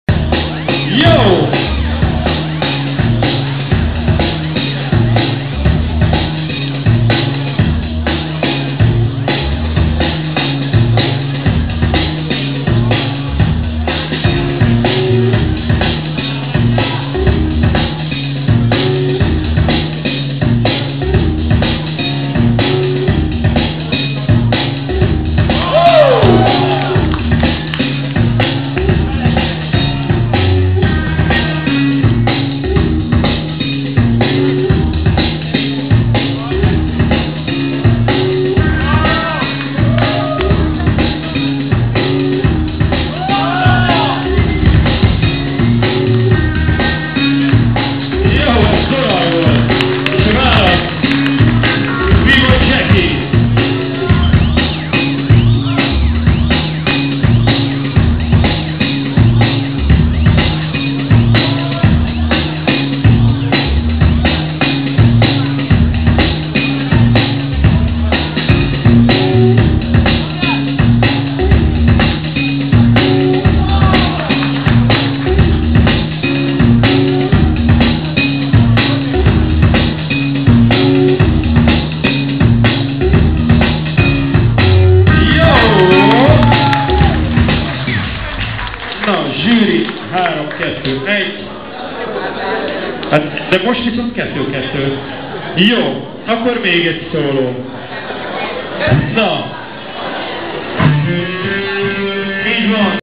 Magyar Breaktánc Bajnokság (Forgotten Realm 1/1) Bboy Jacky Vs. Bboy Vietname Part 1
Magyar Breaktánc Bajnokság (Forgotten Realm 1/1)
Bboy Jacky Vs. Bboy Vietname Part 1
Lurdy Ház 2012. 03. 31.
HD Vágatlan,
Felbontás: 1280 x 720
Formátum: MJPG
Bitráta: 26839 kbps
Képkocka / másodperc: 30.000

by: Λdм™